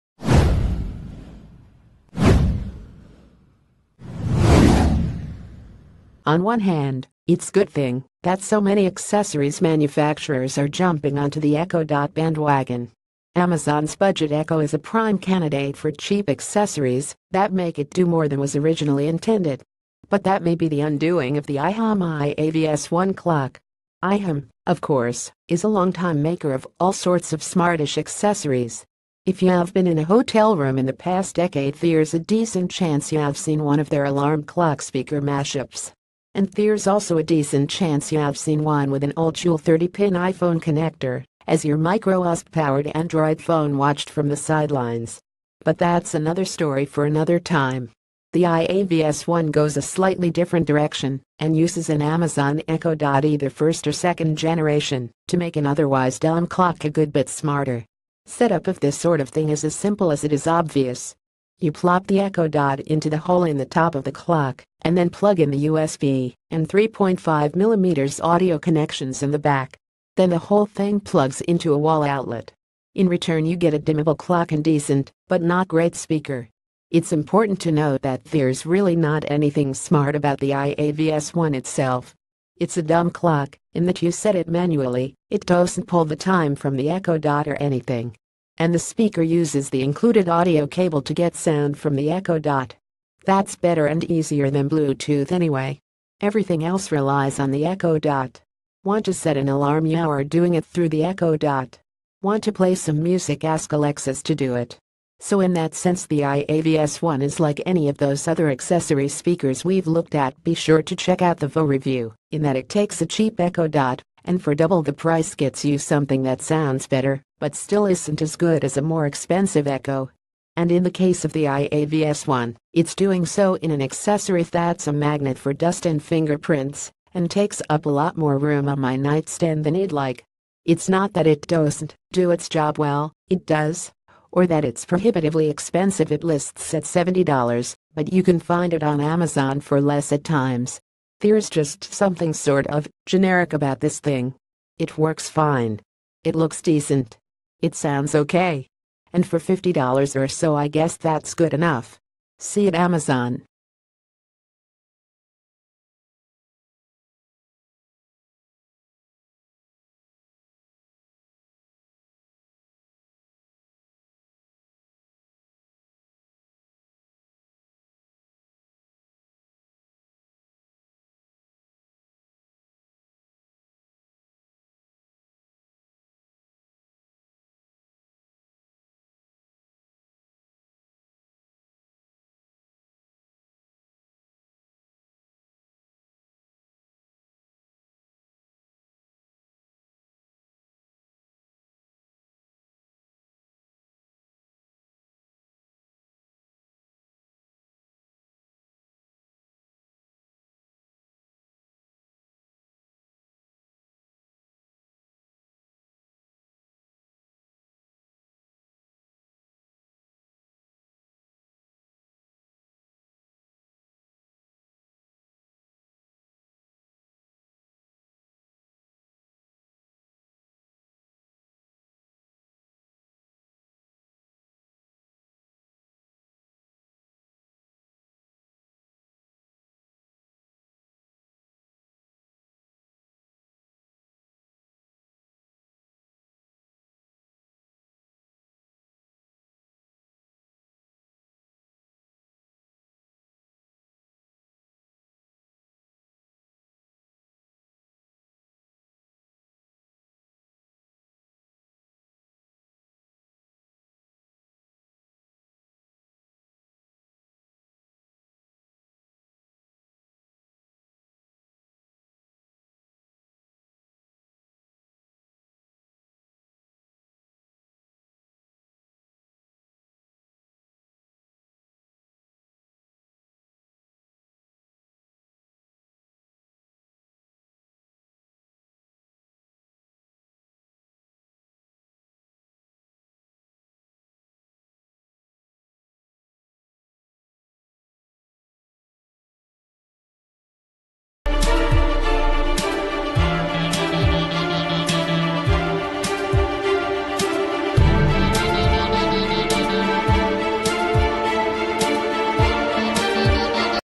Breaking News | The ihome iavs1 is a boring echo dot alarm clock
The ihome iavs1 is a boring echo dot alarm clock

On one hand, its good thing that so many accessories manufacturers are jumping onto the Echo Dot ban...